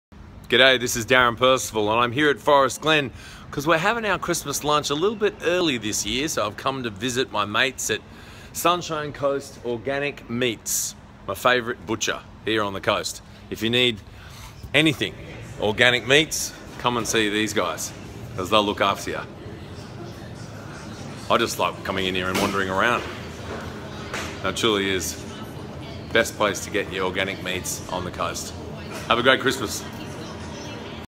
Darren Percival Testimonial
Darren Percival Christmas shopping @ Sunshine Coast Organic Meats